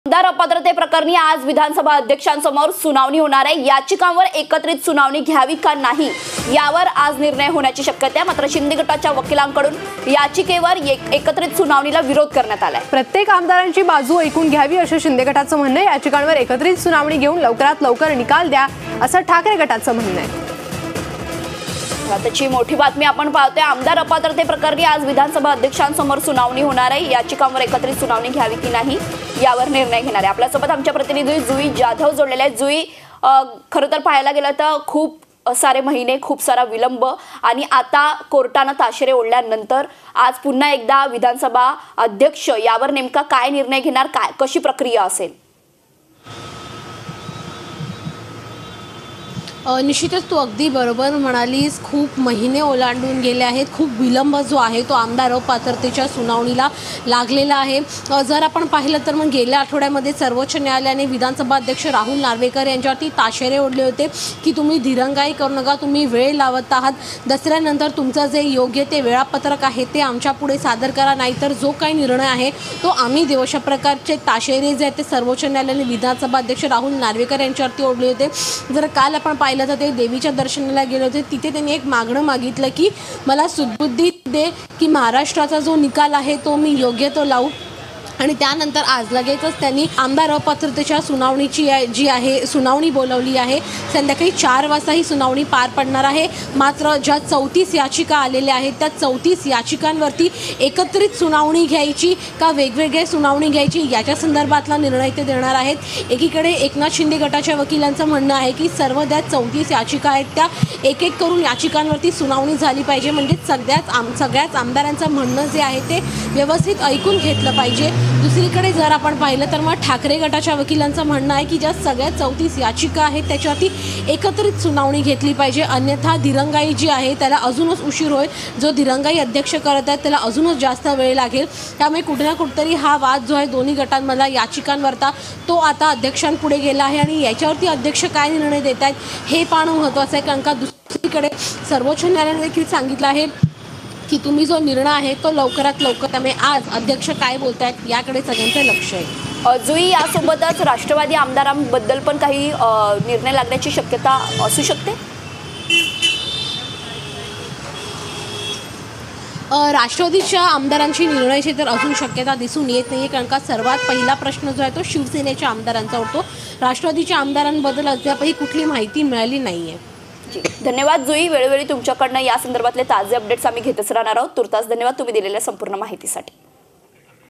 MLA Disqualification: शिंदे गटाकडून याचिकांवर एकत्रित सुनावणीला विरोध, आमदारांची बाजू ऐकून घ्यावी..
Credit: Political|kadam/Producer|vaibhav/Editor